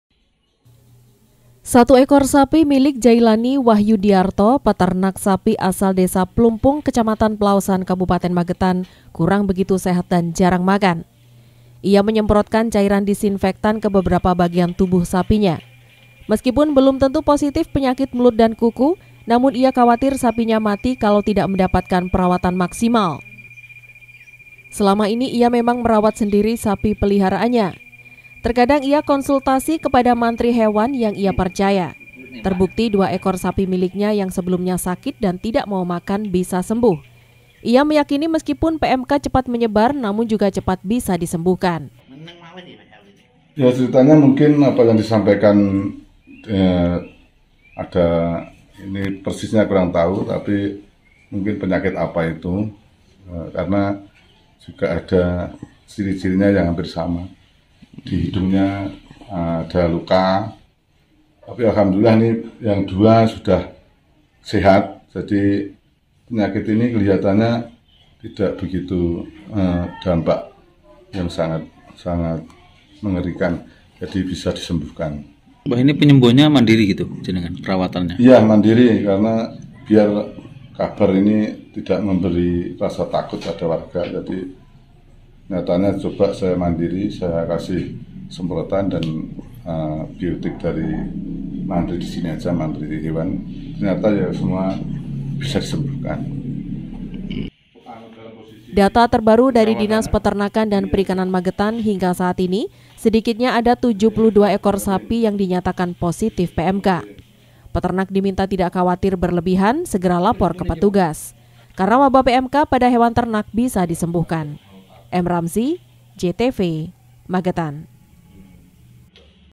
MAGETAN - Antisipasi PMK, Peternak Obati Sendiri Hewan Ternaknya
Meskipun penyebaran wabah pmk pada sapi terus menyebar di kabupaten magetan namun kabar baiknya sapi yang terserang pmk bisa disembuhkan. Peternak di magetan secara swadaya melakuan perawatan terhadap hewan ternak yang sakit atau susah makan.